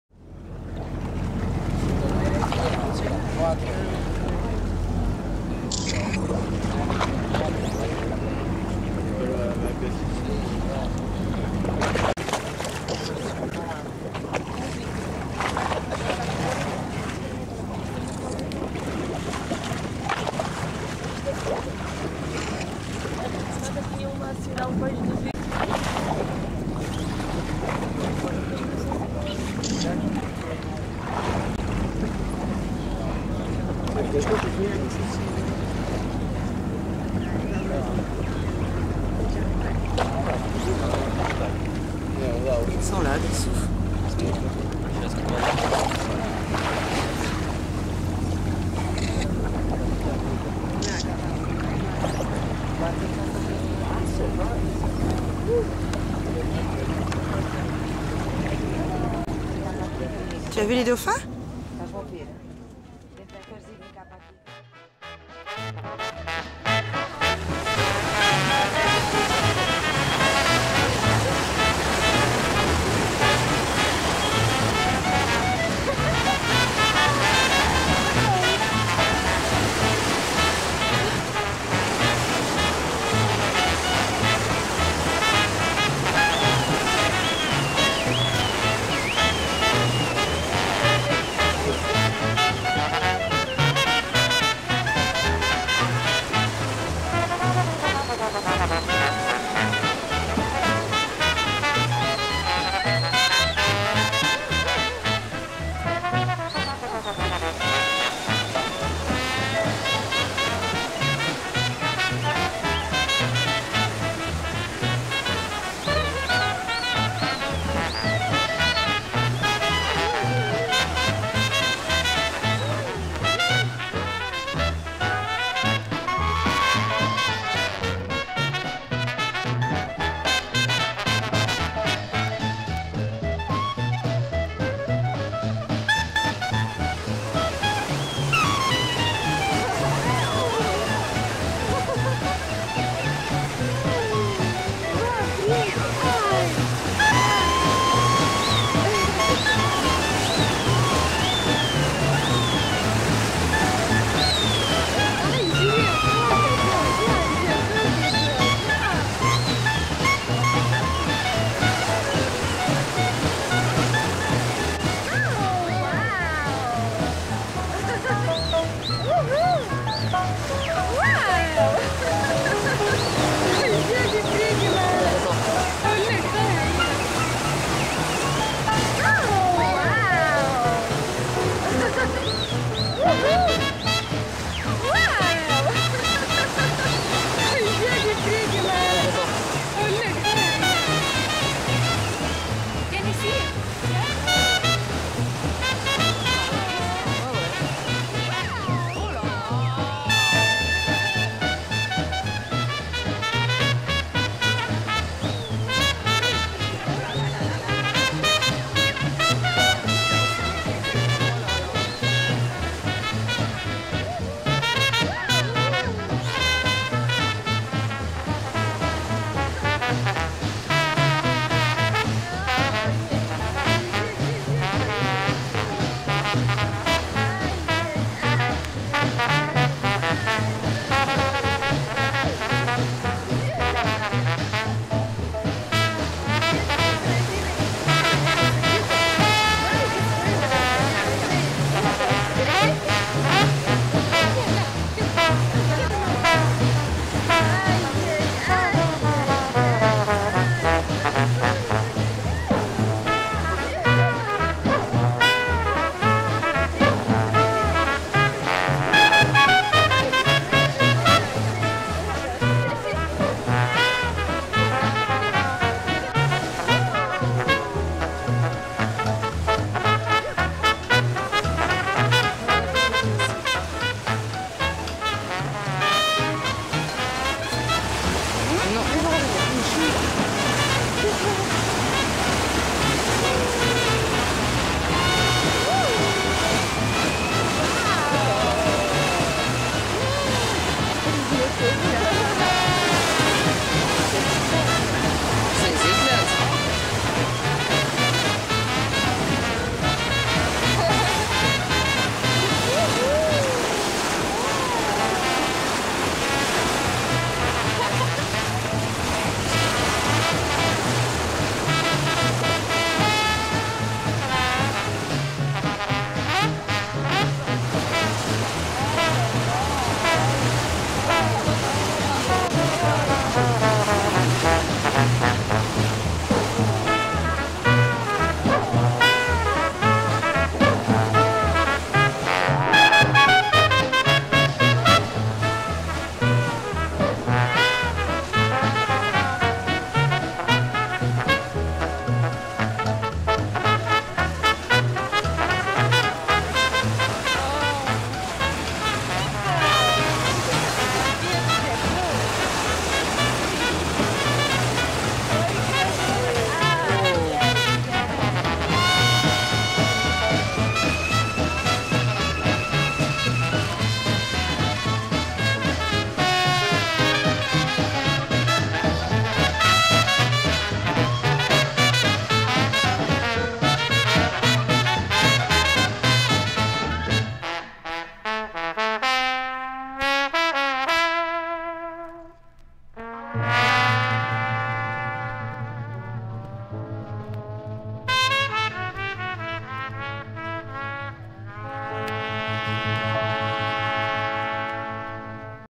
(1674) TENERIFE - Deux ARTISTES .mpg
La rencontre de deux dauphins et de deux Jazzmen...
Des frères : Les deux dauphins... Les deux Assuntos et les deux touristes penchés sur le rebord du bateau et qui n'en perdent pas une miette !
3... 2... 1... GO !